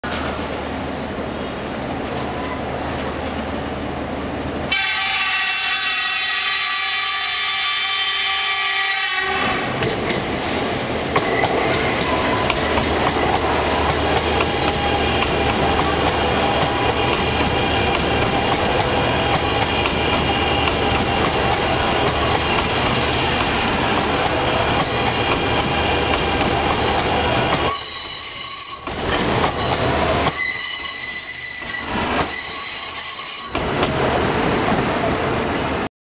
CRH2C, China High Speed Railway 鄭西高鐵 (G2009鄭州至西安, ZhengZhou to XiAn Train)
On 2010, ZhengZhou to XiAn High Speed train CRH2C, Train No G2009, arrived XiAn Station.
2010年, G2009次列车, 鄭州至西安CRH2C高鐵, 到達鄭西高鐵西安站.